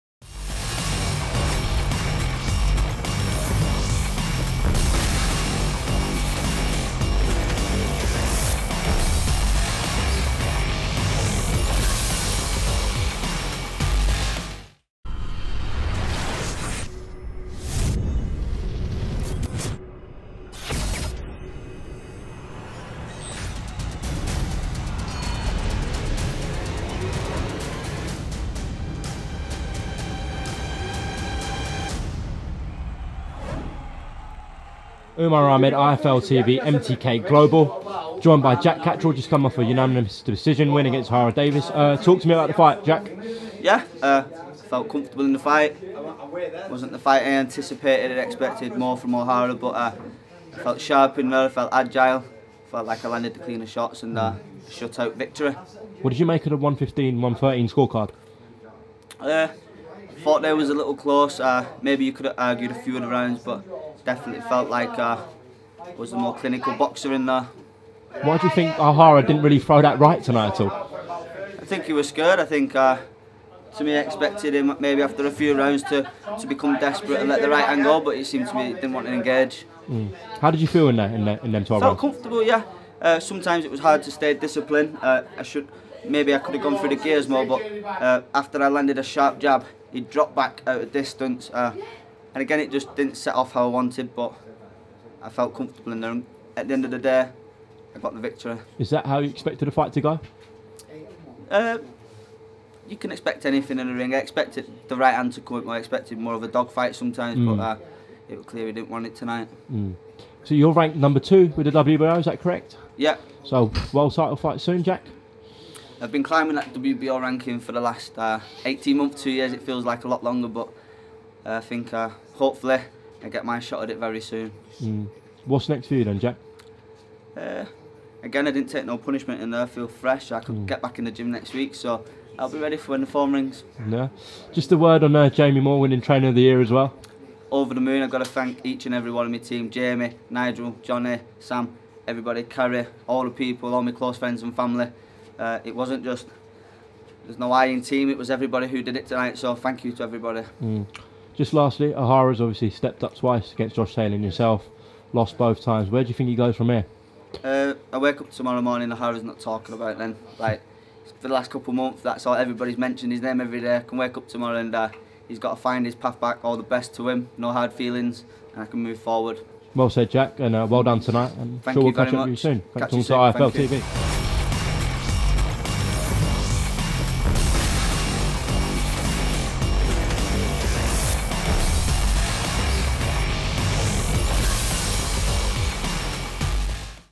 ‘I EXPECTED MORE OF A DOG FIGHT, HE DIDN’T WANT IT TONIGHT’ - CATTERALL REACTS TO BEATING DAVIES
POST-FIGHT INTERVIEW BY UMAR AHMED

Don’t miss the action – tune into BoxNation, Sky Sports and BT Sports and watch the fights live. Not signed up? You’ll find the best offers for new customers here: